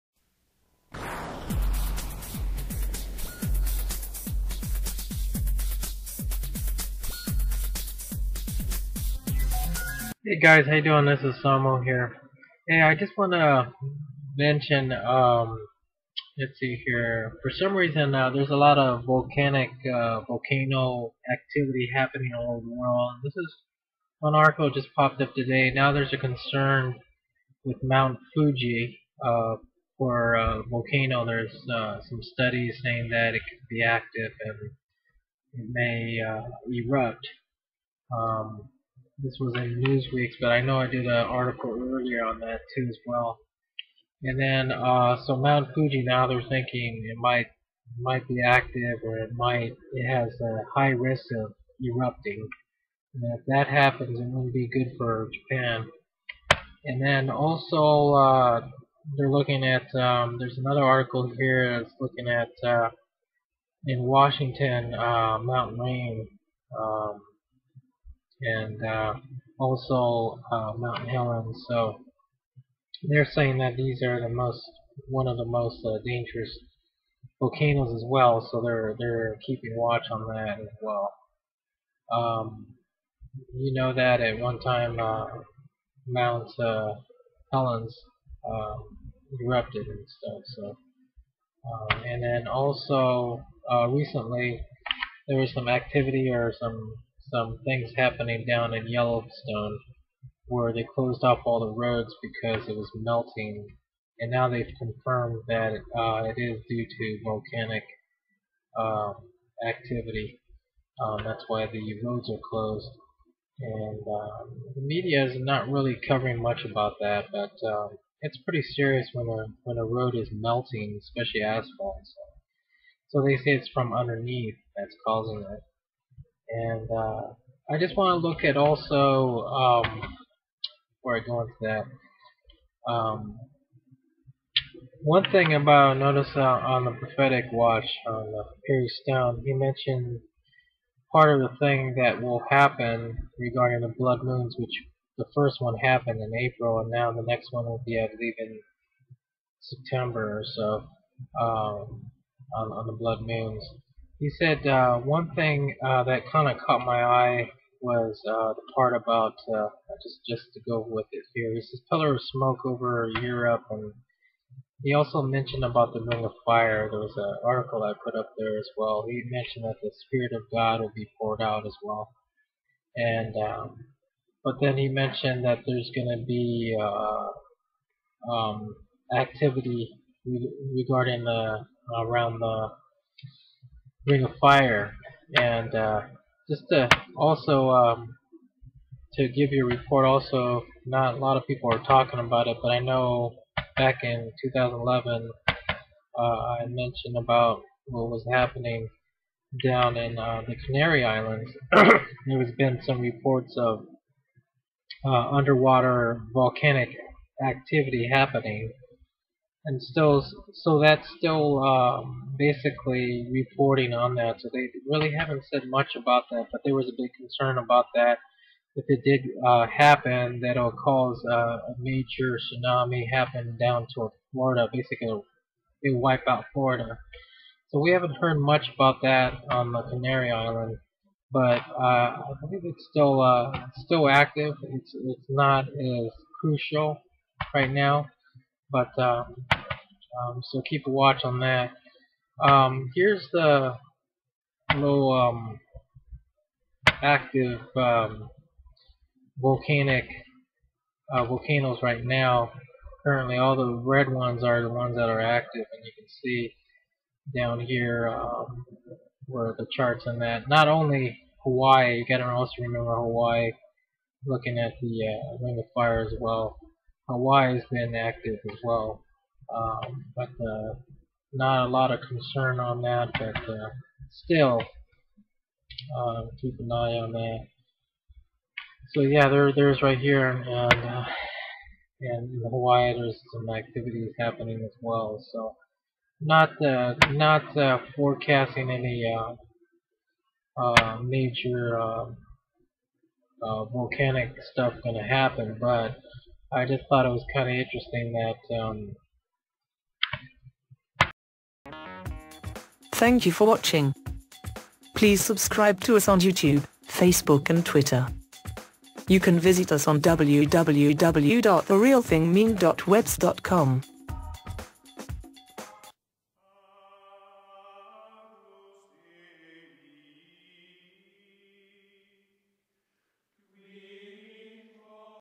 Volcano Alerts Mt Fiji, Yellowstone,Mt Ranier, St Helens, El Hierro!
More Active Volcano happening!
Mount Fiji, Mount St Helens, El Herrio, Hawaii